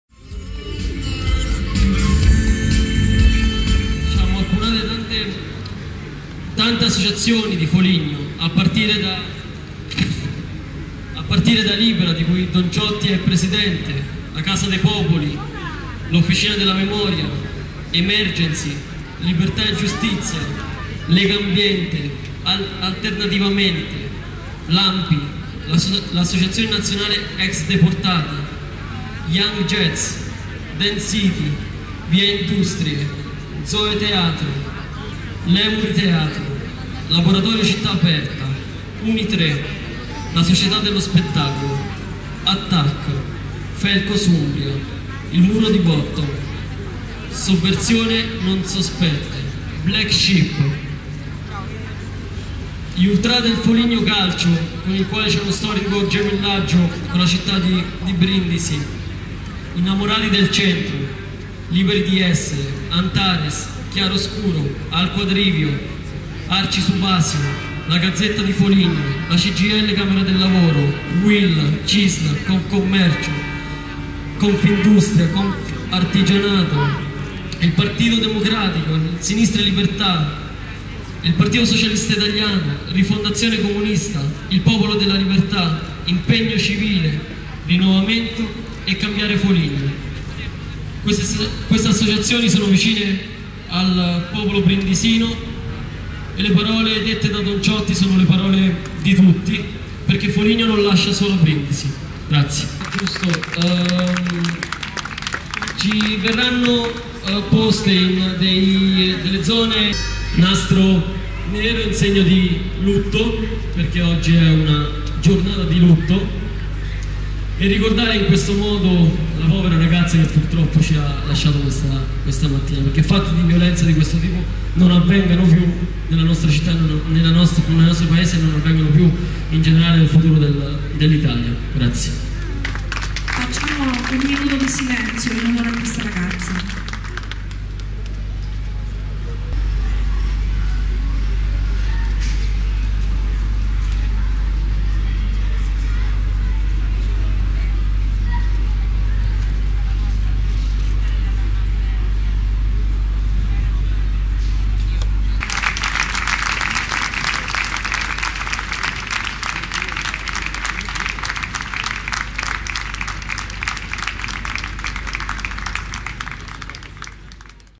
Notte Rosa Foligno 2012 clip1
Notte Rosa Foligno 2012 segnata dal terribile attentato a Brindisi. La partecipazione lo sdegno e la volontà a non farsi intimorire espresse dai giovani, da tutte le associazioni, l'amministrazione i partiti politici di Foligno. Letto un comunicato di Don Ciotti fondatore di Libera.  ''Proviamo un grande dolore, tanto dolore e vogliamo innanzitutto esprimere tutta la nostra vicinanza alle famiglie e a tutti i ragazzi della scuola. Bisogna certo aspettare l'esito delle indagini sull'attentato. Quello che ora sentiamo di poter e dover dire che una morte di questo genere e' inaccettabile. In Puglia ci sono beni confiscati alle mafie - ha aggiunto Ciotti - dove tanti giovani si danno da fare per ridare a questo nostro paese piu' legalita', piu' dignita', piu' lavoro, piu' giustizia sociale''. ''Lo stesso avviene in tante scuole della Regione e del paese dove i ragazzi come quelli colpiti oggi dall'attentato - prosegue - imparano non solo le materie del sapere ma anche l' alfabeto della cittadinanza e della corresponsabilita'. Che questo fatto violento, incredibile, non puo' farci dimenticare la meraviglia di questi ragazzi impegnati a costruire il loro ma anche il nostro futuro''.